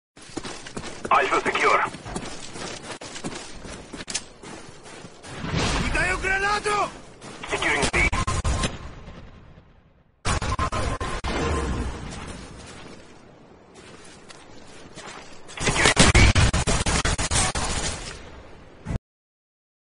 Quad kill to Open Game
Playing on my friends account and PS3 as opposed to my xbox, i nicely lobbed a grenade towards the gate for a first blood quad.